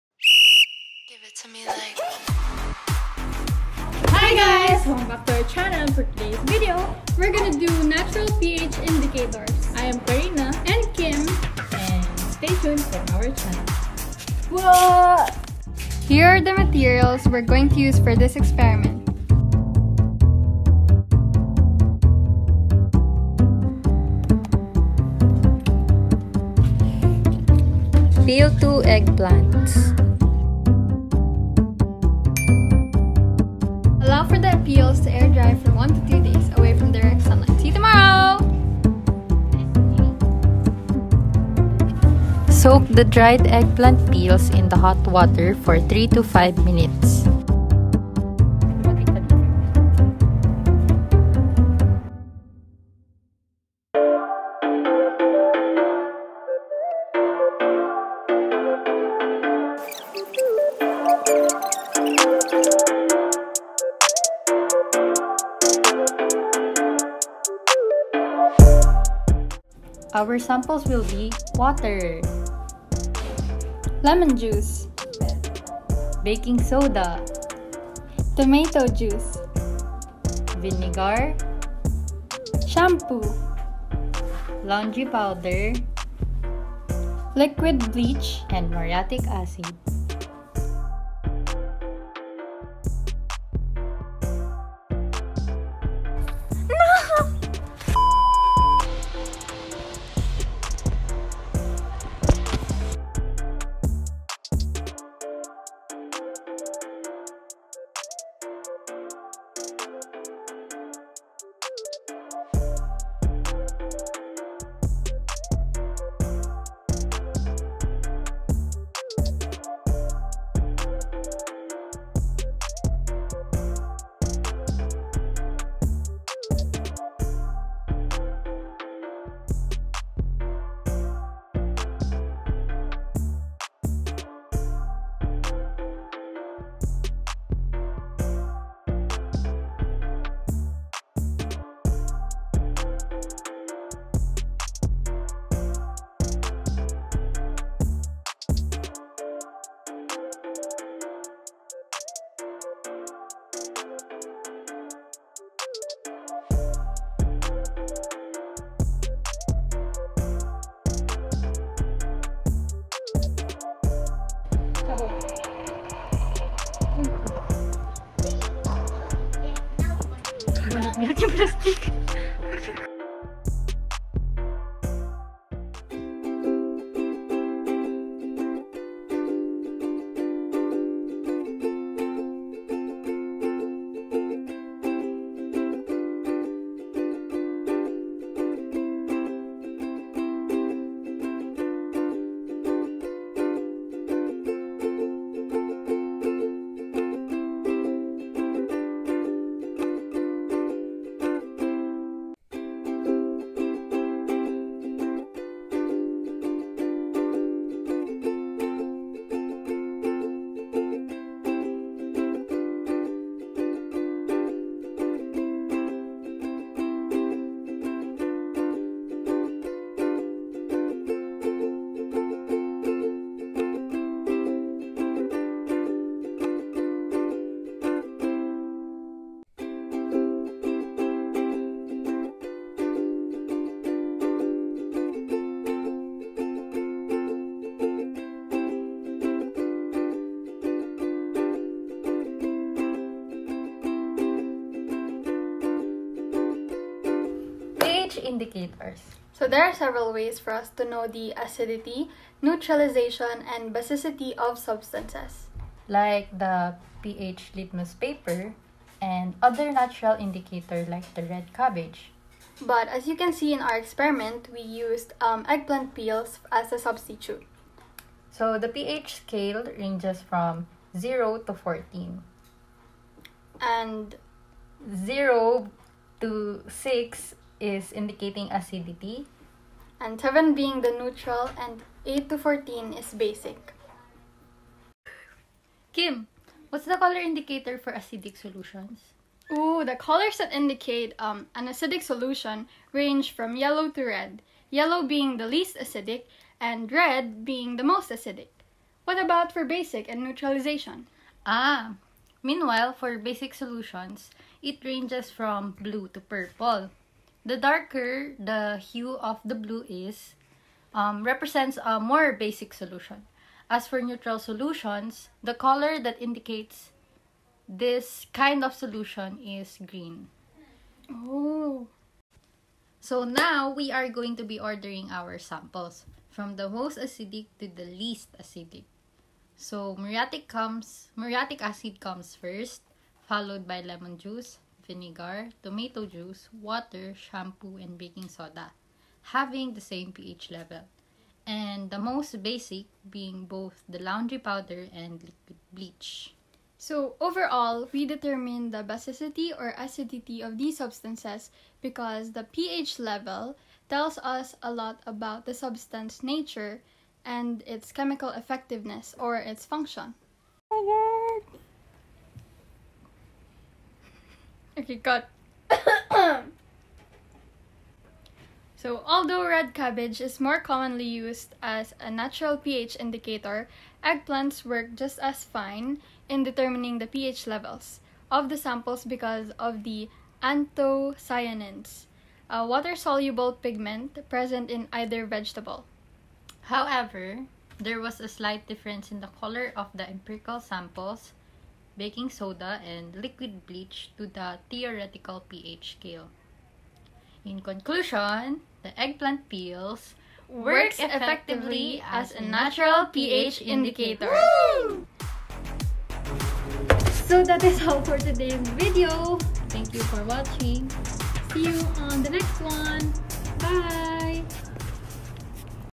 Natural pH Indicators: Eggplant Peels
Karina De Guzman and Kim Ilagan

What color was the indicator for acidic solutions? What about for basic and neutral solutions?

The colors that indicate an acidic solution range from yellow to red. Yellow being the least acidic and red being the most acidic. Meanwhile for basic solutions it ranges from blue to purple. The darker the hue of the blue is, represents a more basic solution. As for neutral solutions, the color that indicates this kind of solution is green.

What was the order of your samples, from most acidic to least acidic?

In order of most acidic to least acidic solutions, the muriatic acid comes first, followed by lemon juice, vinegar, tomato juice, water, shampoo and baking soda, having the same pH level, and the most basic being both the laundry powder and liquid bleach. Overall, we determine the basicity or acidity of these substances because the pH level tells us a lot about the substance’s nature and tells us its chemical effectiveness or its function.

What is the organic compound responsible for the color changes observed? What is the basis for these color changes?

In the process of preparing the natural pH indicator, we steeped the eggplant peels in boiling water for an ample amount of time. During this time being, an organic compound called anthocyanins dissolved into the water. This accounts for the changes in color because when the molecule is exposed to a certain pH environment, it changes its molecular structure. The changes are visibly seen in the color they appear as in terms of the pH scale.

Red cabbage is the usual plant source for this kind of experiment. Why do you think this experiment still worked even though you used eggplants?

Although red cabbage is more commonly used as a natural pH indicator, eggplants would work just as fine because of the anthocyanins, a water-soluble pigment, present in either vegetable. As was mentioned previously, anthocyanins are the main factor in the changes in color, being exposed to either acidic or basic solutions changes the molecular structure of the anthocyanin. Thus, absorbing light that we perceive in different colors depending on the acidity or basicity of the solution.

Conclusion

PH indicators: there are several ways to know the acidity, neutralization, and basicity of substances. Like the pH Litmus paper, and other natural indicators like the red cabbage indicator, but in this case, we used eggplant peels as a substitute. The pH scale ranges from 0 to 14. 0 to 6 indicating the solution is acidic, 7 being neutral and 8 to 14 is basic. However, there was a slight difference in the color of the empirical samples like baking soda and liquid bleach, from the theoretical pH scale. In conclusion, eggplant peels work effectively as a natural ph indicator.